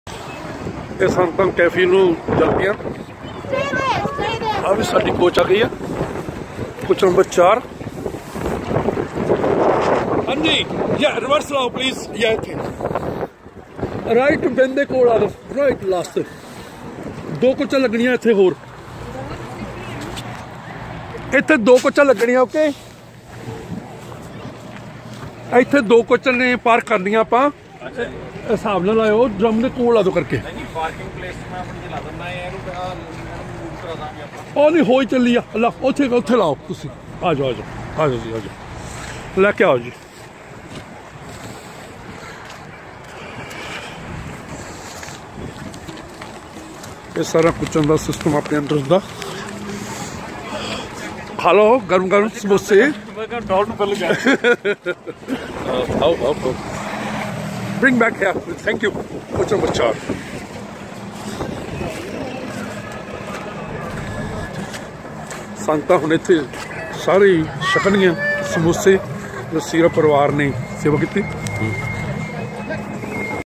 Baba Balak Nath Ji Yatra 45 in UK by Gorkha Malhi 2018 9-26
On Mela Baba Balak Nath Ji 45 Yatra From Southall to Walsall Mandir Baba Balak Nath Ji 96a Caldmore Road,Walsall,West Midlands, WS1 3PD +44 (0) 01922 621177